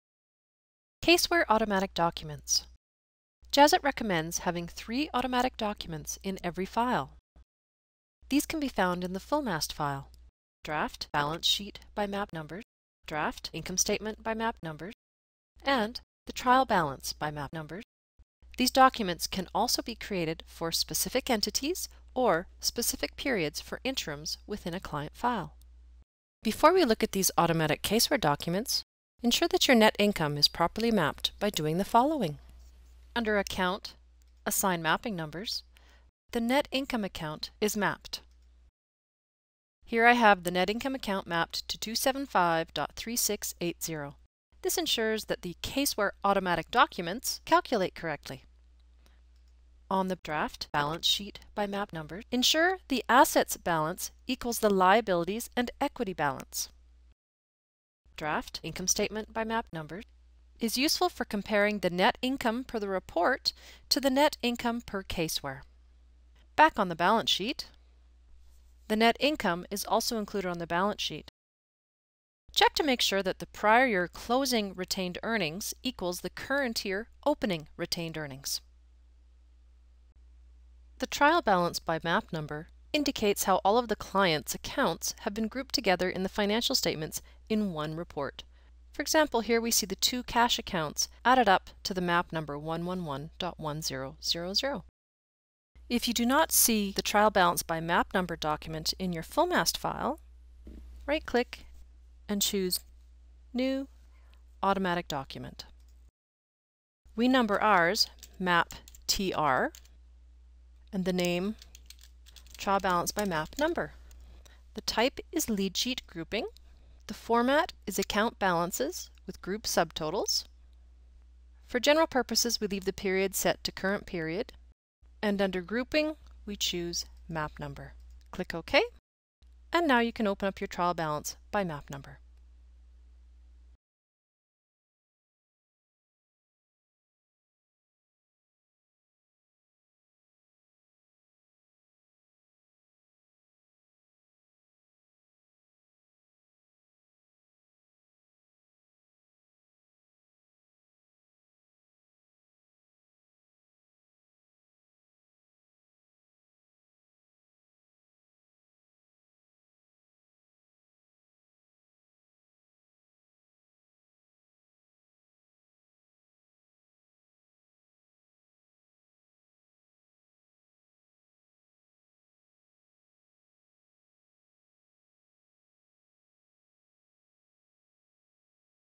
Jazzit Top Tip #7: CaseWare automatic documents
We recommend you include three CaseWare automatic documents on your client document manager.  These are:

1. Trial balance by map number
2. Draft balance sheet by map number
3. Draft income statement by map number